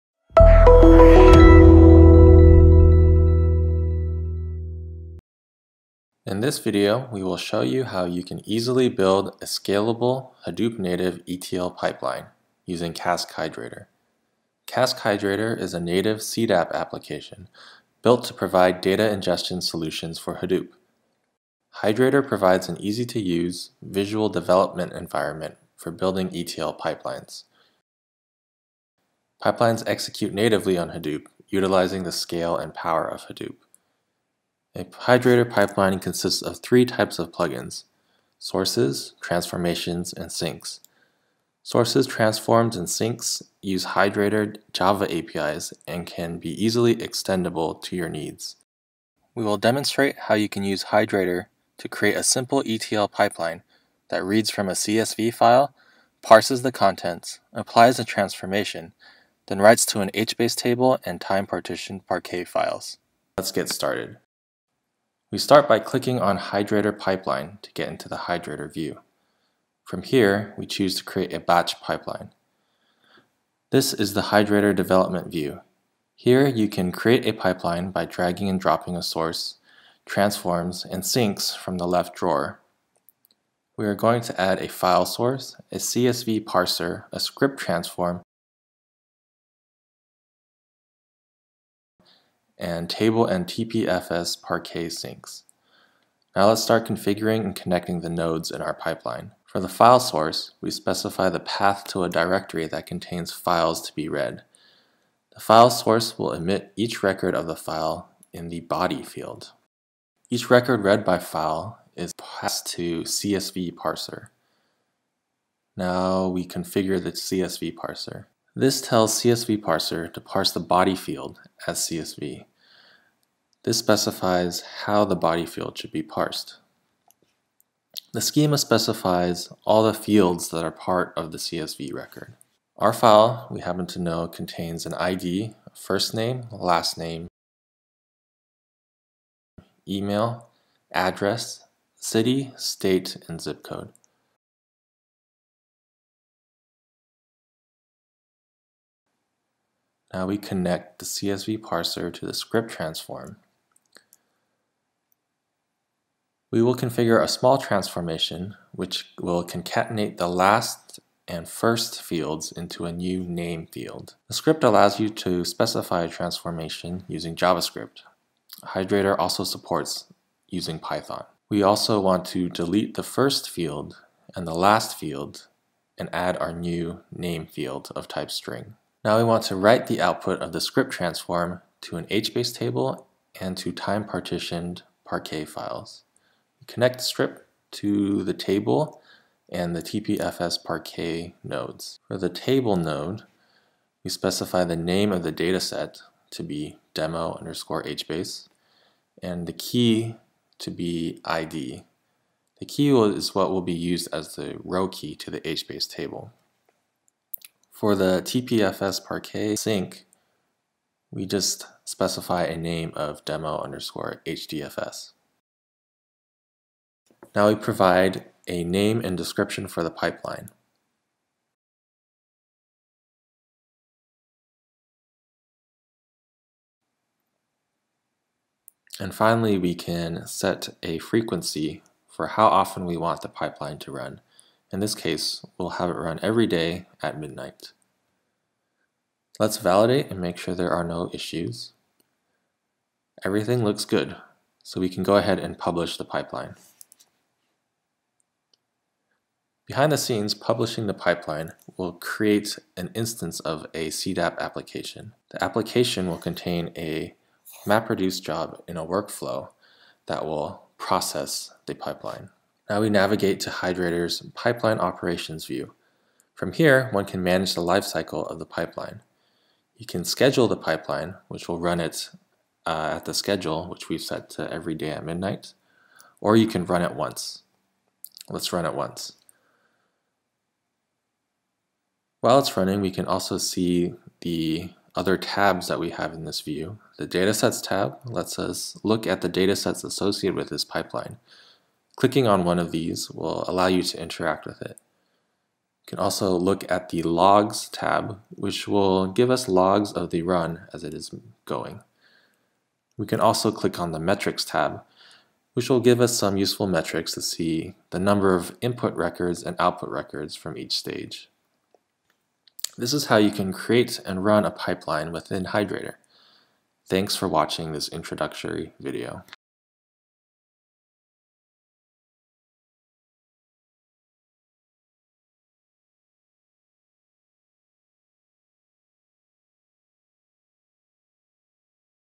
Introducing Cask Hydrator
Cask Hydrator is a highly functional framework and UI to support self-service batch and real-time data ingestion, and ETL. Hydrator provides CDAP users a code-free way to configure, deploy, and operationalize ingestion pipelines from different types of data sources.